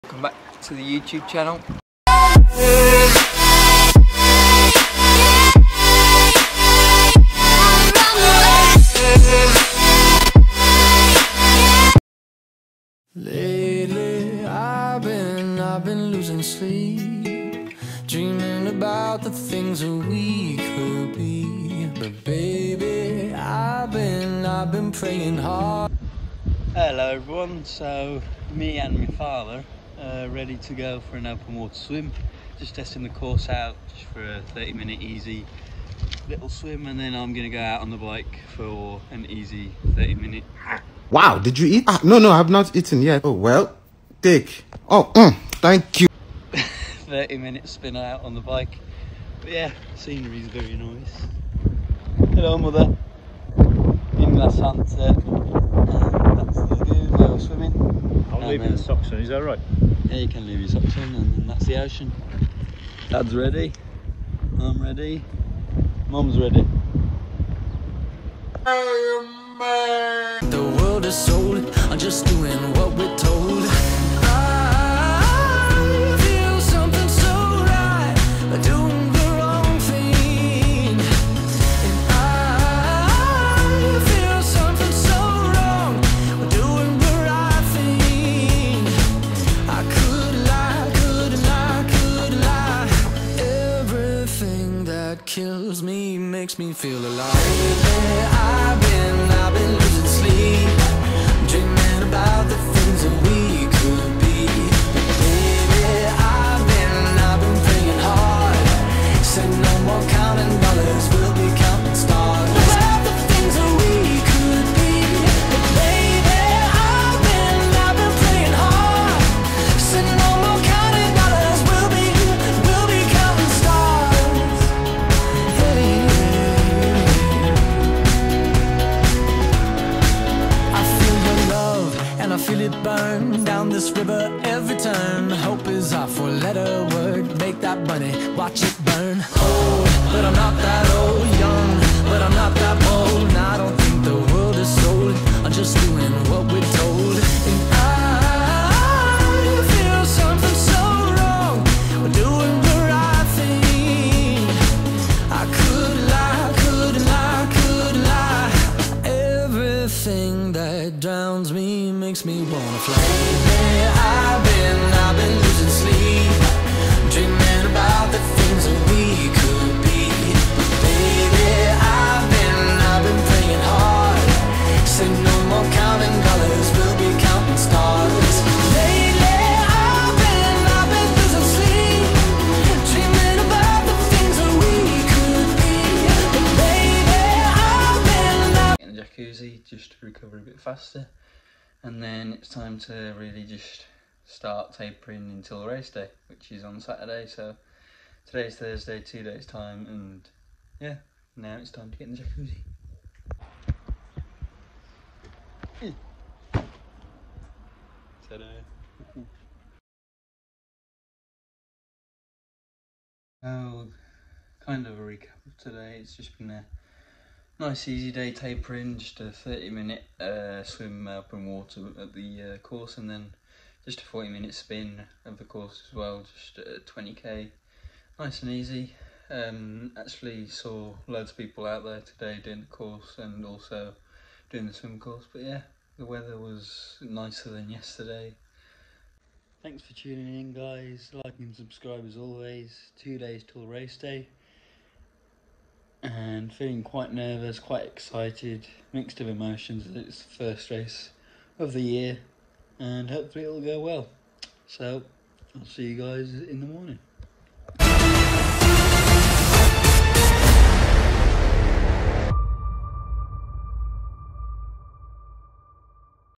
Lanzarote Volcano Triathlon Episode 3
Another day done, just easy training today,